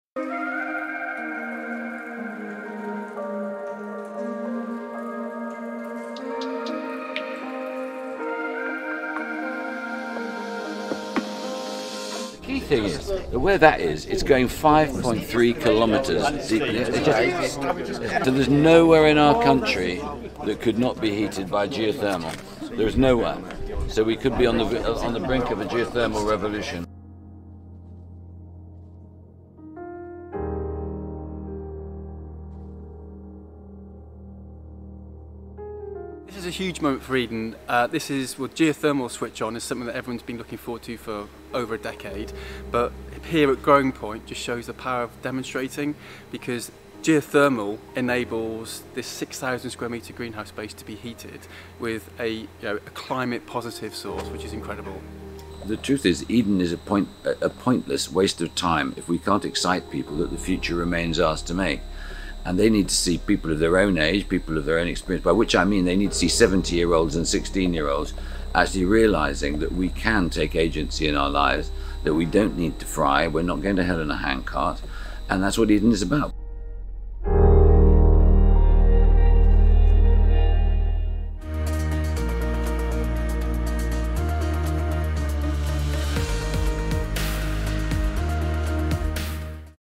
Eden Geothermal is UK’s first operational deep geothermal project to come online in 37 years
A landmark moment for renewable energy took place at the Eden Project in Cornwall on Monday as the UK’s first operating deep geothermal heating plant since 1986 was opened and is now generating heat.

The process of drilling deep into the granite at the home of the world-renowned Biomes in Cornwall was finished in 2021, and Eden Geothermal Ltd (EGL) has now completed the heat main and plant and is ready to supply heat to the Eden Project and its new state-of-the art nursery, Growing Point.

You can get all the latest news, sport, showbiz and comment at Cornwalllive.com